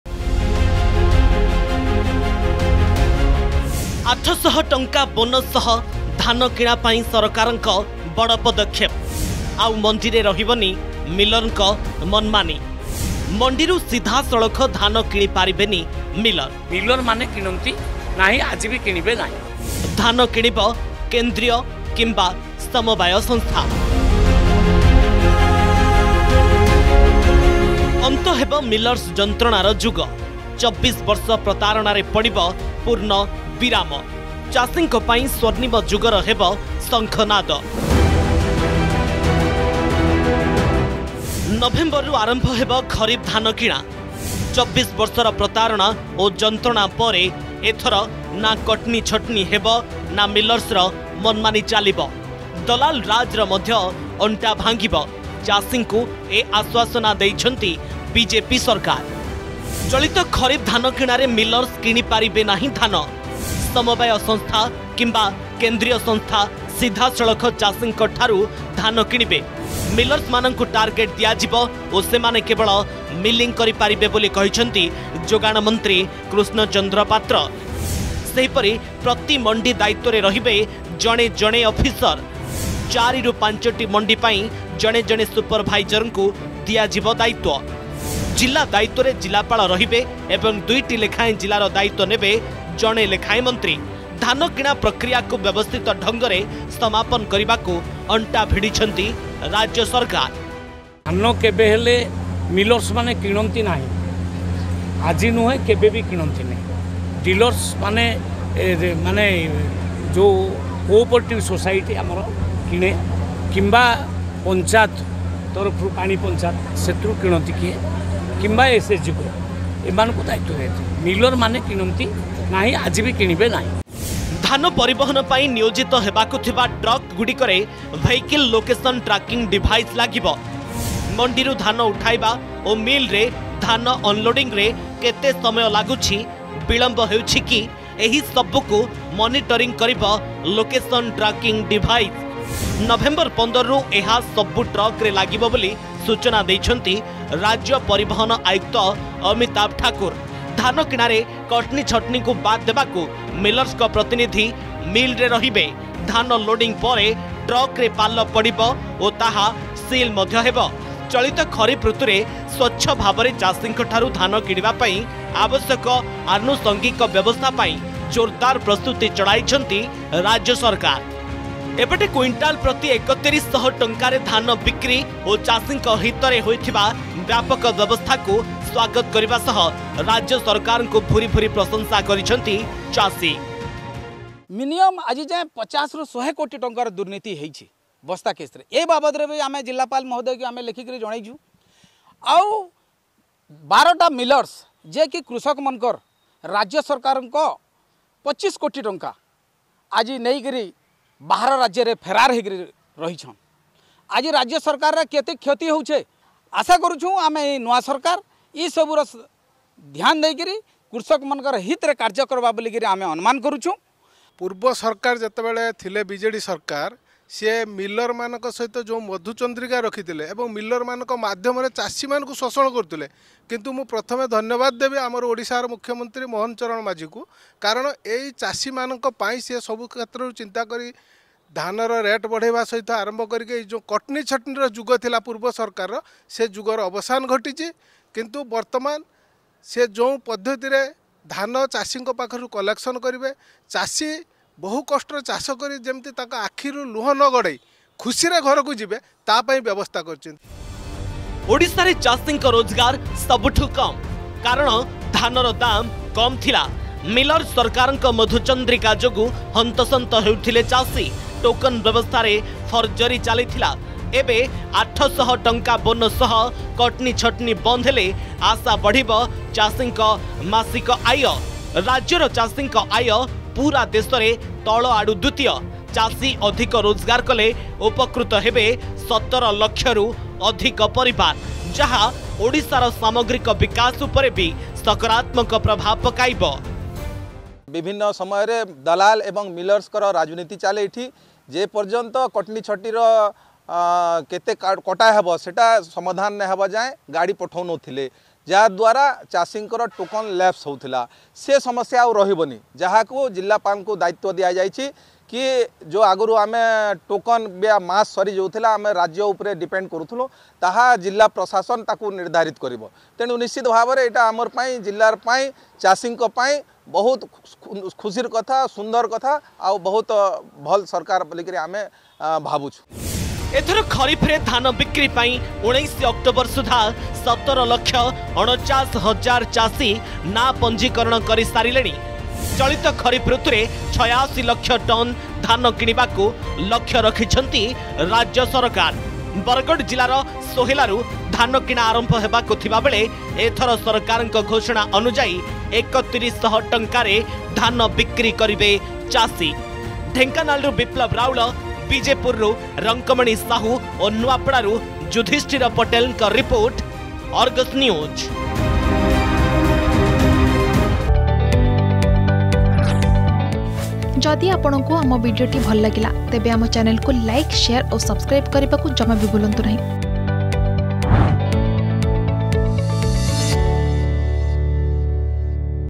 ଆଉ ମଣ୍ଡିରେ ରହିବନି ମିଲରଙ୍କ ମନମାନୀ | Katni Chatni Issue
ଅନ୍ତ ହେବ ମିଲର୍ସ ଯନ୍ତ୍ରଣାର ଯୁଗ । 24 ବର୍ଷ ପ୍ରତାରଣାରେ ପଡ଼ିବ ପୂର୍ଣ୍ଣଚ୍ଛେଦ । 800 ଟଙ୍କା ବୋନସ୍ ସହ ଧାନ କିଣା ପାଇଁ ବିଜେପି ସରକାରଙ୍କ ବଡ଼ ପଦକ୍ଷେପ । ଆଉ ମଣ୍ଡିରେ ରହିବନି ମିଲରଙ୍କ ମନମାନି । ସିଧାସଳଖ ଧାନ କିଣି ପାରିବେନି ମିଲର । କେନ୍ଦ୍ରୀୟ କିମ୍ବା ସମବାୟ ସଂସ୍ଥା ଚାଷୀଙ୍କଠାରୁ କିଣିବେ ଧାନ ।